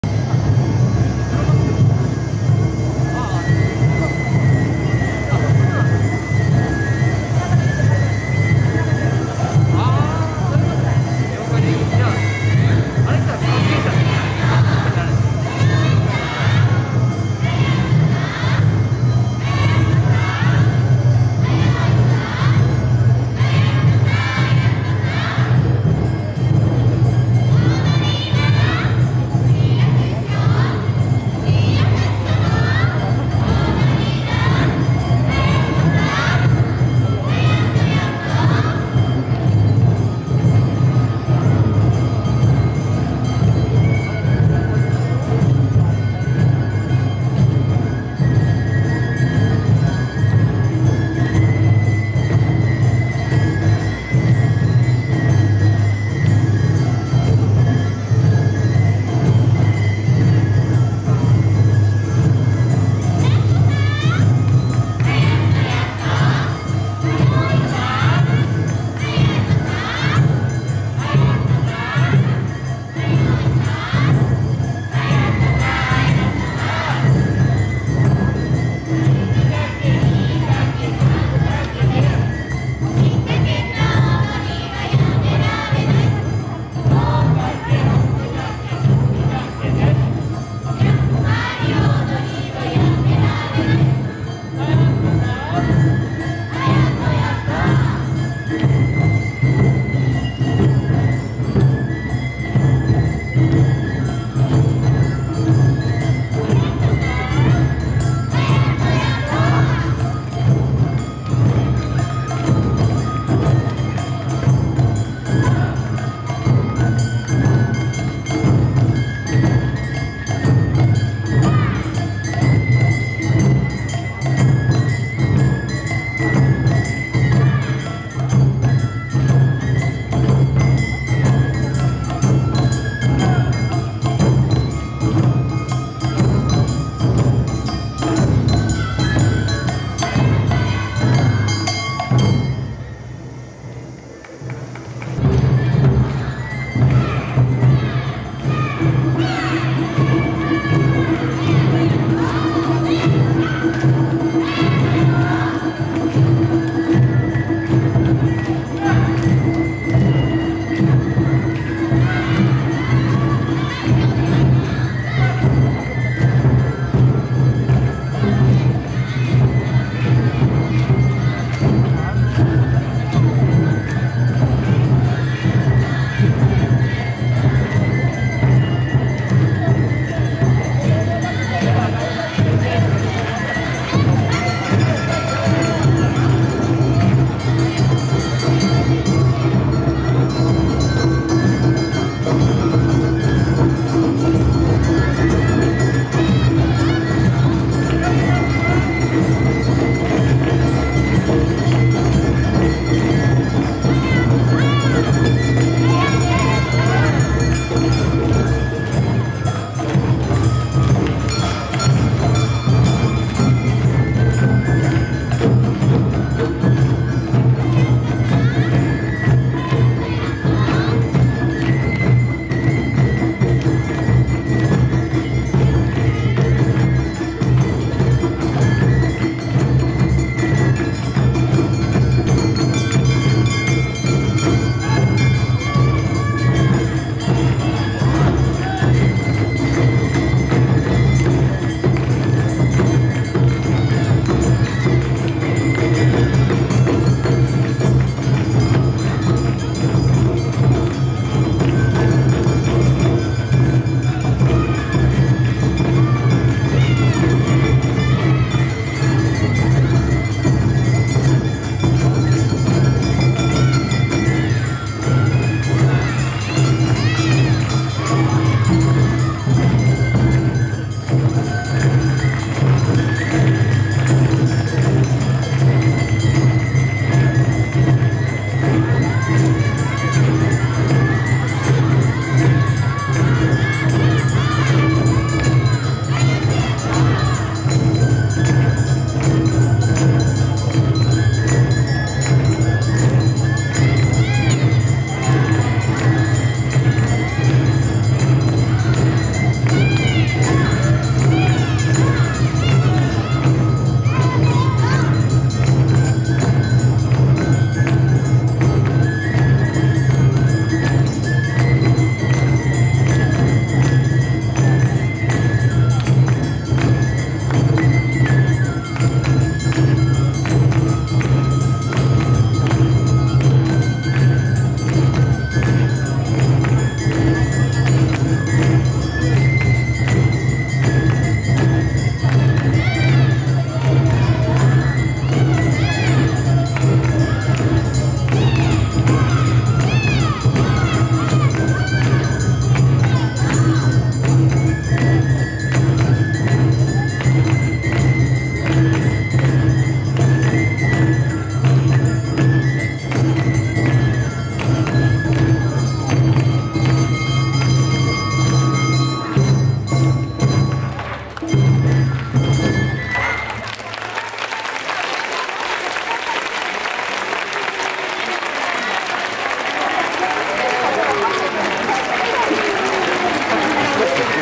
みたか市役所連🕺Re；Start 三鷹阿波おどり～はじめの一歩～ 2022✨Awa Odori (Awa Dance)⭐2022年(令和4年)8月21日(日)😇21 August, 2022(Sun)
「三鷹阿波おどりＲｅ；Start～はじめの一歩～」
■開催日時　令和4年8月21日（日）午後4時～6時
■開催場所　三鷹駅南口 中央通り🔷東京都三鷹市下連雀3-37-15
💃🏼三鷹阿波踊り振興会